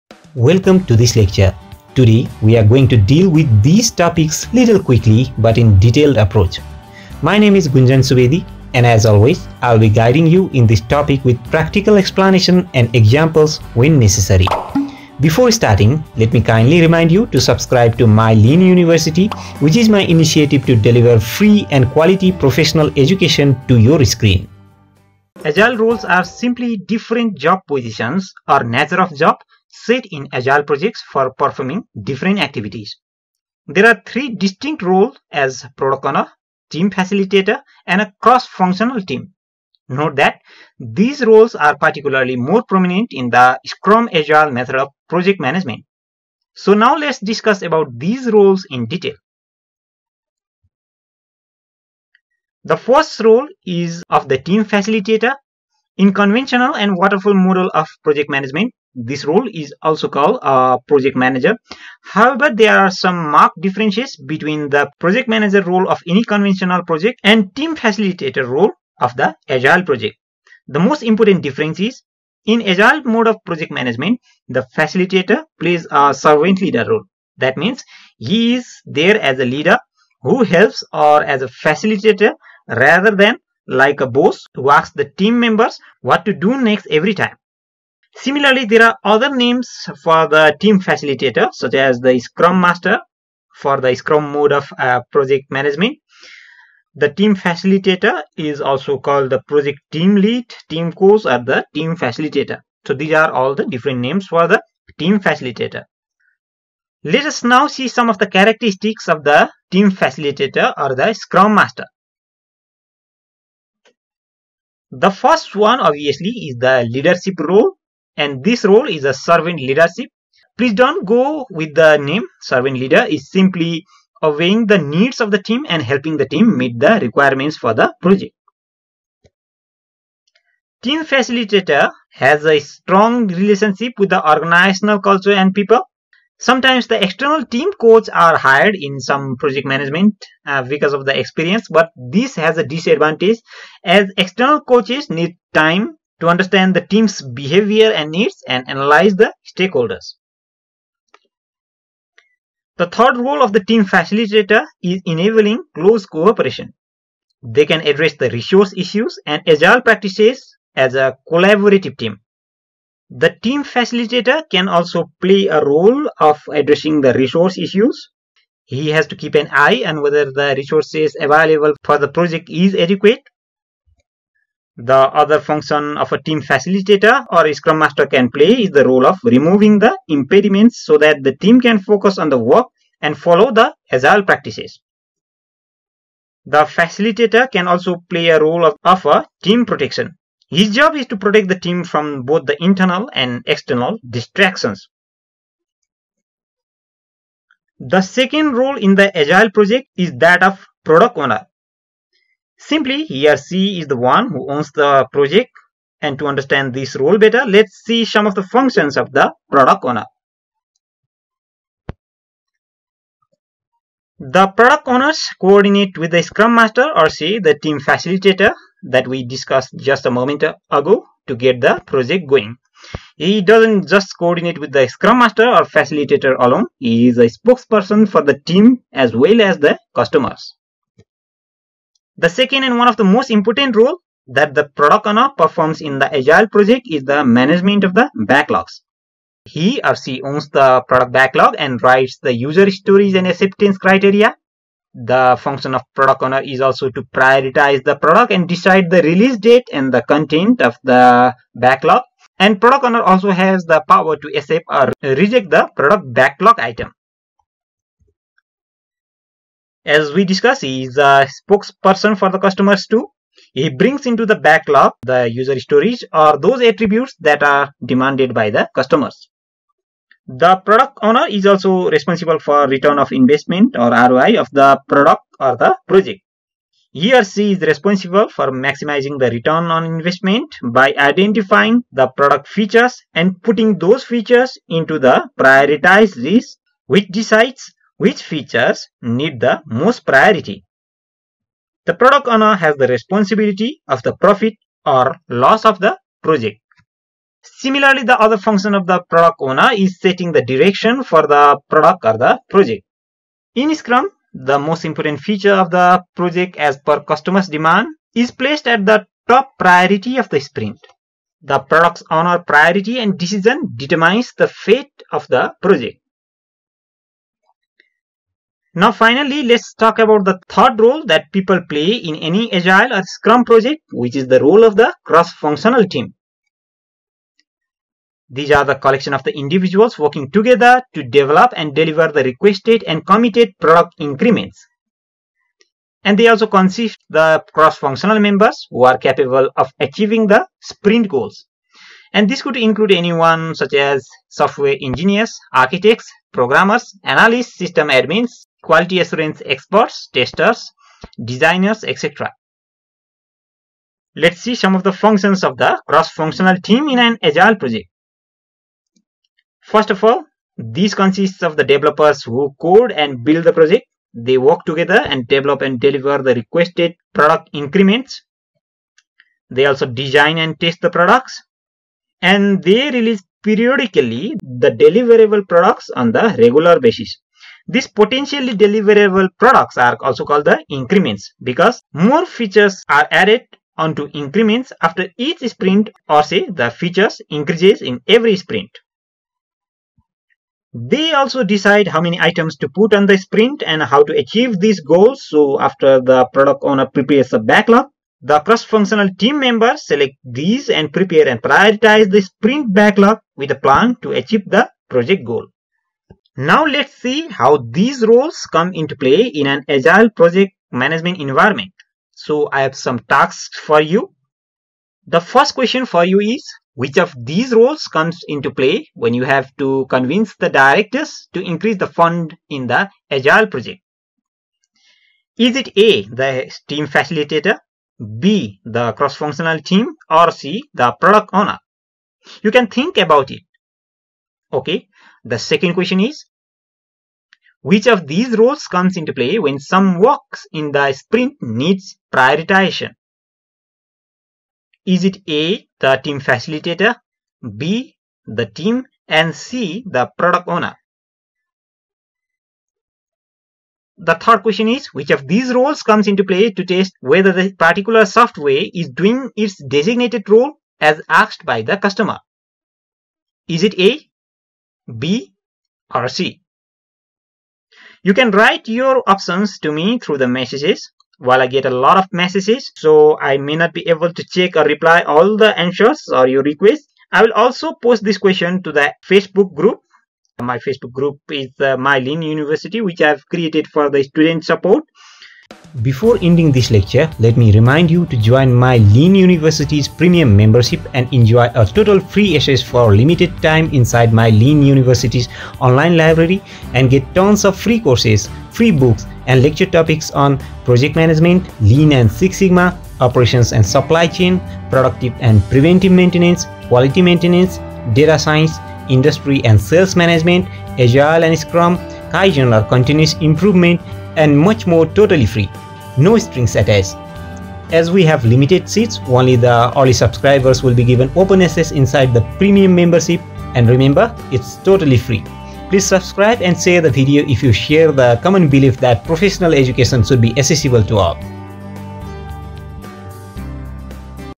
Agile Roles I Product Owner , Team Facilitator , Cross Functional Team
In this video, we will study about different agile members or agile roles in detail. We will study in brief about,
Product Owner
Team Facilitator and
Cross Functional Team